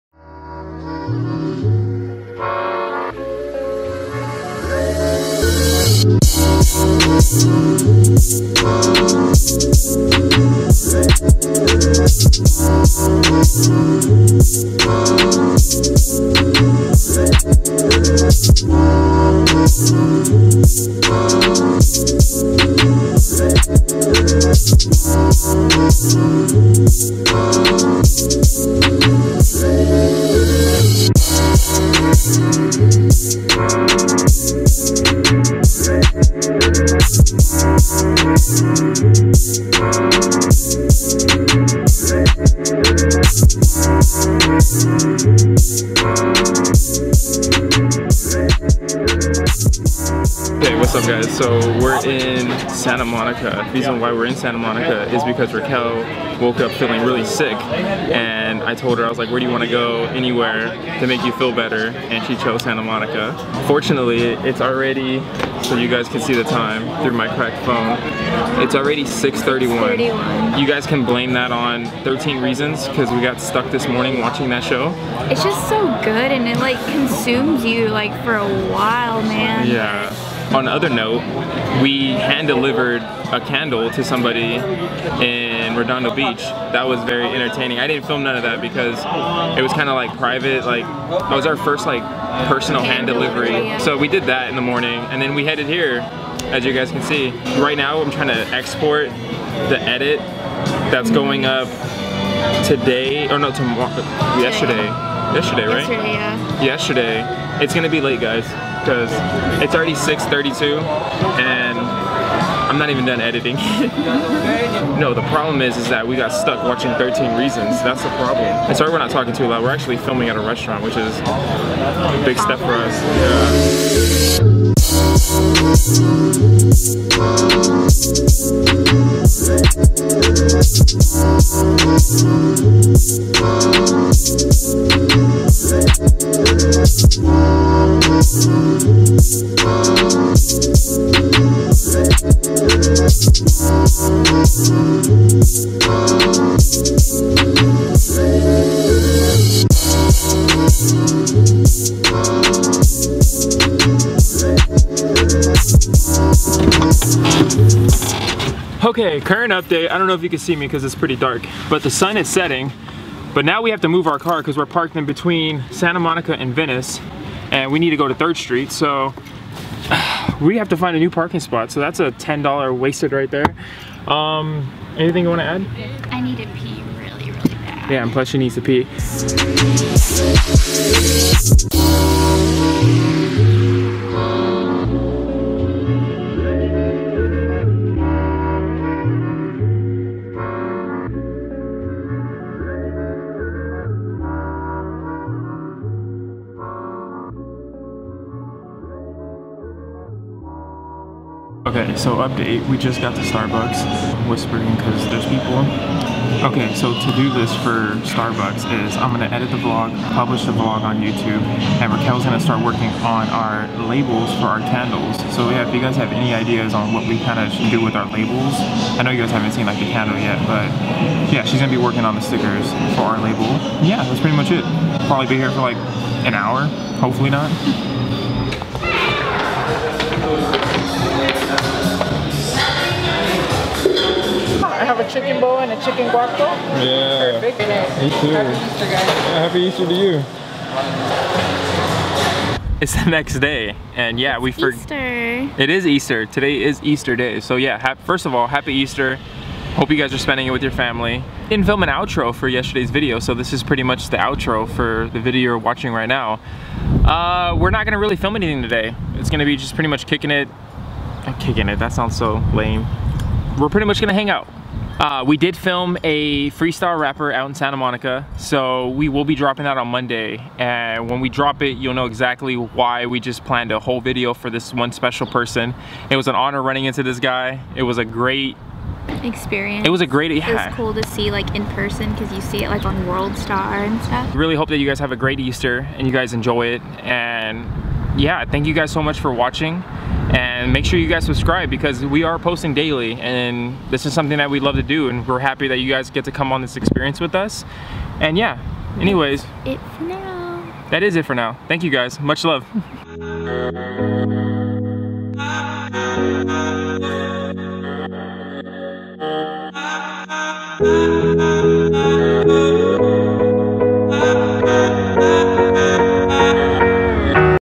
A BEACH A DAY KEEPS THE DOCTOR AWAY!
SANTA MONICA! IN TODAYS VIDEO ME AND RAQUEL TOOK A TRIP DOWN TO THE BEACH AND RELAX.

MUCH LOVE FOR TAKING THIS EXPERIENCE WITH US!